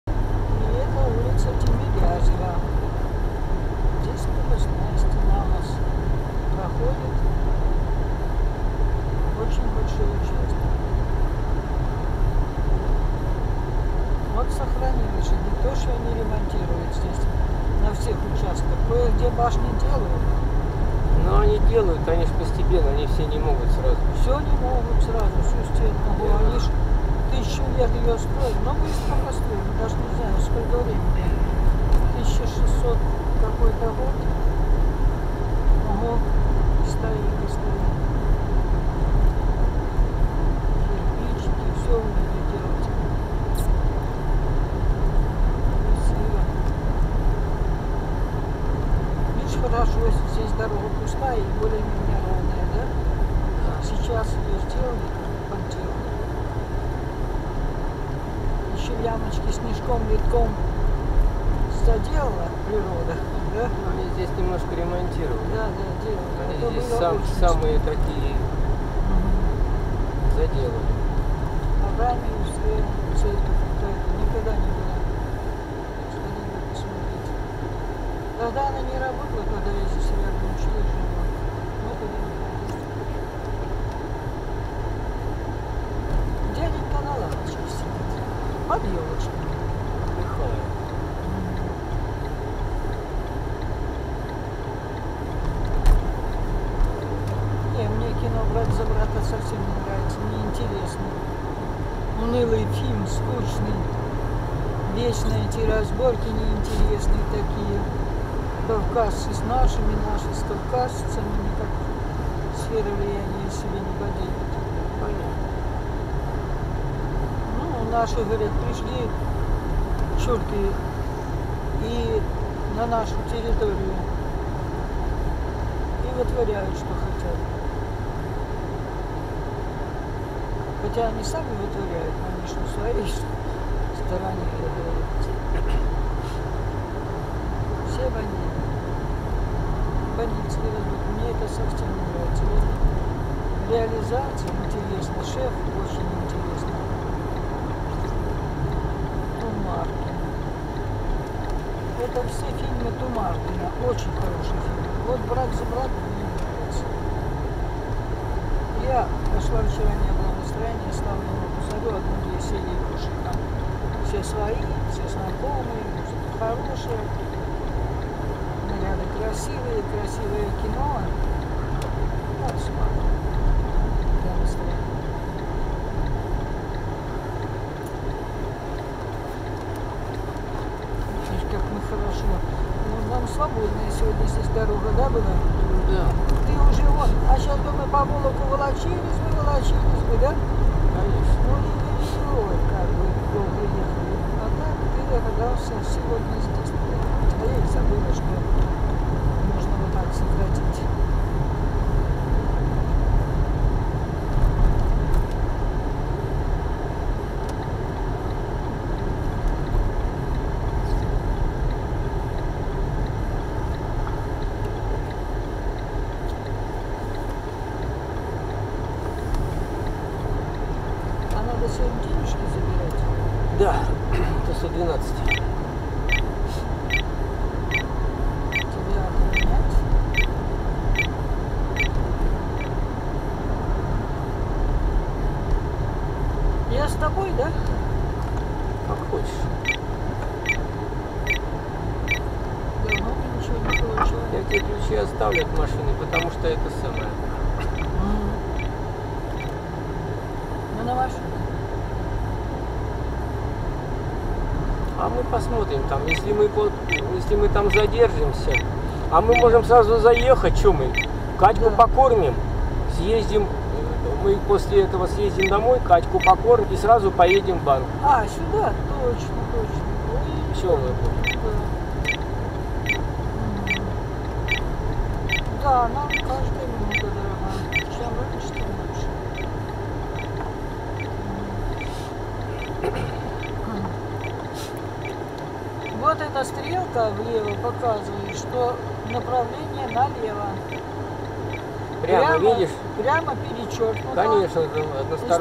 Как проехать. Улица Тимирязева до улицы Исаковского.
Как проехать. Улица Тимирязева до улицы Исаковского. Смоленск. Проезд перекрестков.